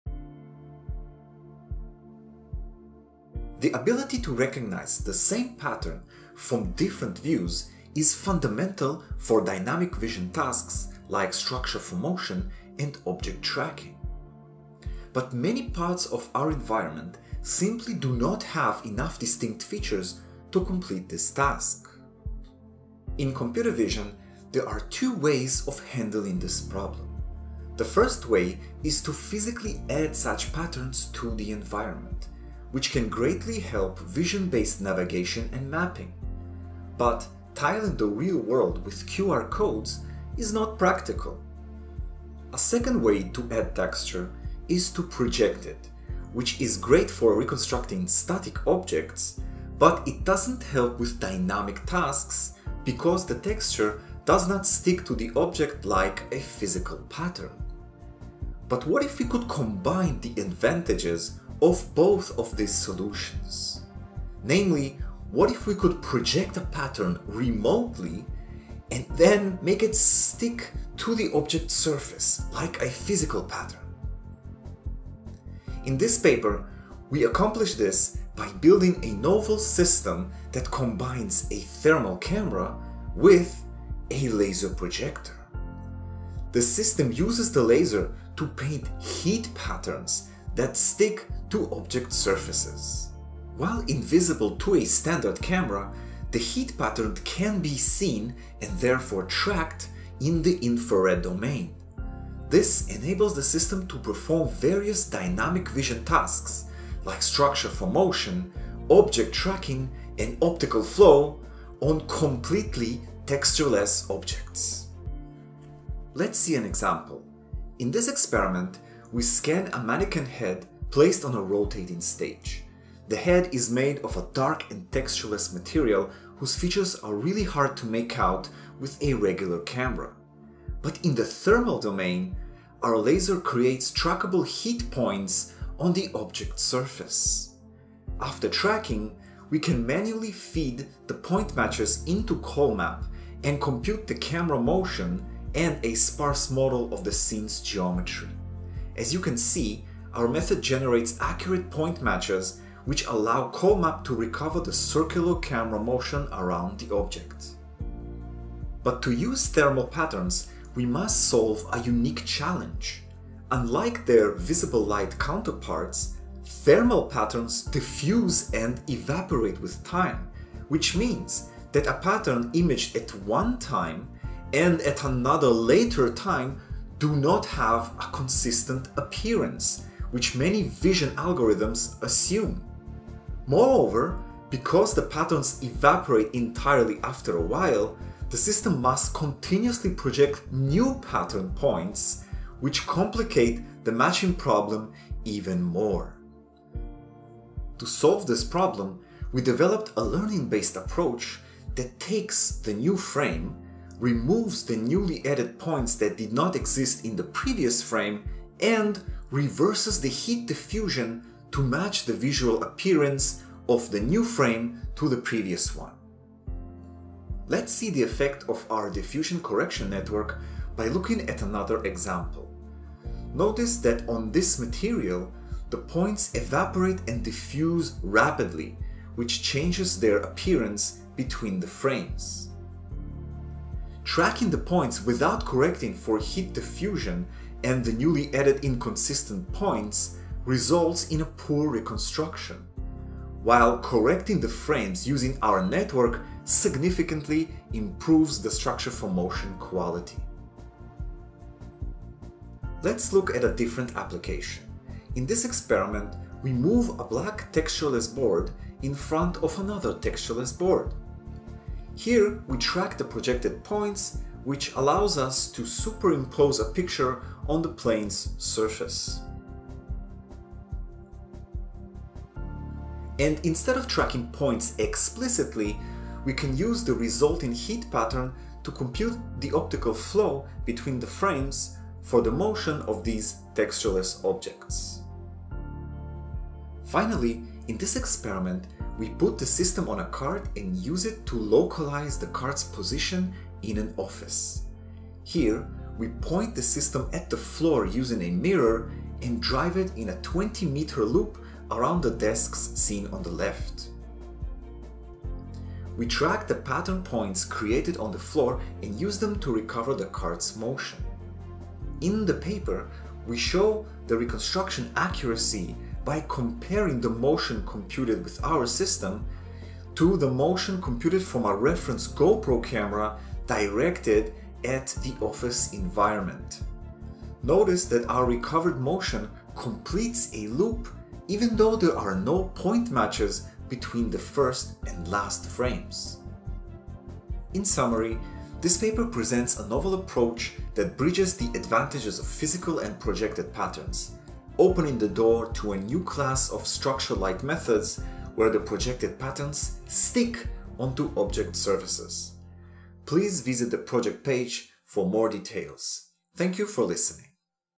Projecting Trackable Thermal Patterns for Dynamic Computer Vision (CVPR 2024)
TL;DR Abstract: We present a novel imaging system that can "paint" and observe its own custom patterns on scene object surfaces. The painted patterns are heat patterns created by steering a laser at the scene and observed by a thermal camera. The new system is useful for dynamic vision tasks like structure from motion, object tracking, and optical flow in challenging textureless environments.

M. Sheinin, A. Sankaranarayanan, and S. Narasimhan. "Projecting Trackable Thermal Patterns for Dynamic Computer Vision," In Proc. IEE/CVF CVPR 2024.